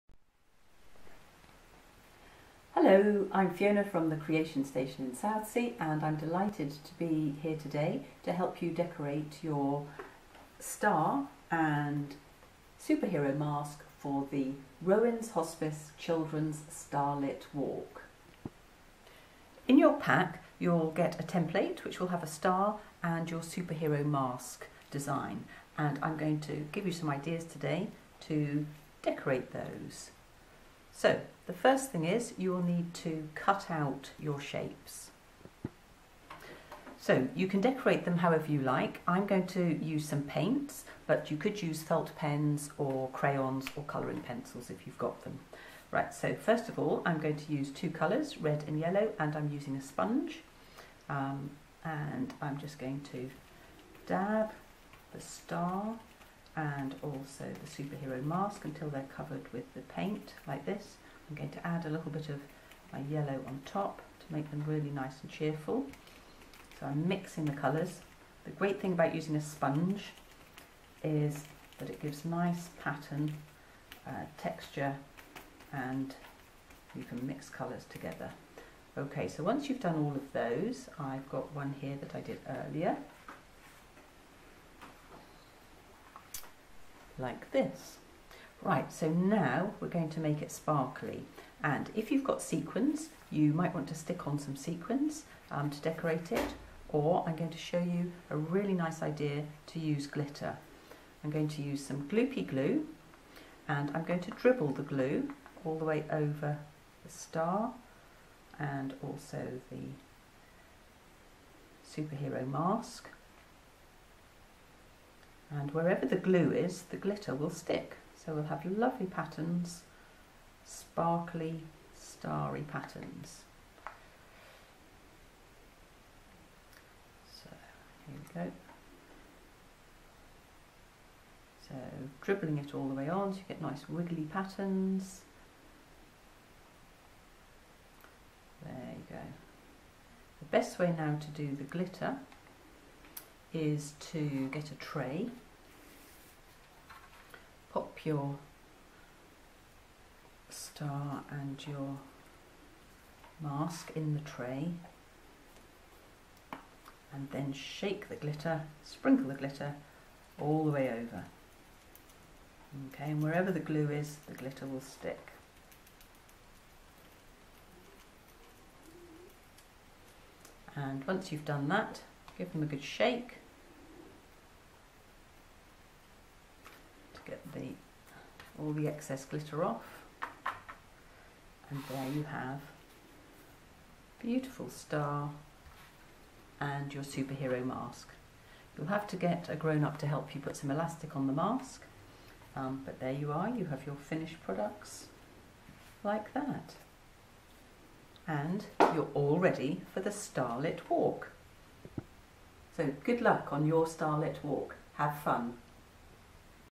Make Your Own Memory Star for Children's Starlit Walk
Get ready to sparkle on the Rowans Hospice Children's Starlit Walk!

Make a glittery superhero mask to wear on the way round, and shiny Memory Star to hang on the memories board in St Ann's Church, using the templates you got in your fundraisers pack.

The walk is happening on 27th October at 6pm, and will take you on a tour of the Portsmouth naval dockyard which isn't normally open to the public!

For more information on how to get your pack and register take part in the walk, go

Many thanks to Creation Station (Portsmouth) for making this video! See more children's arts and crafts videos: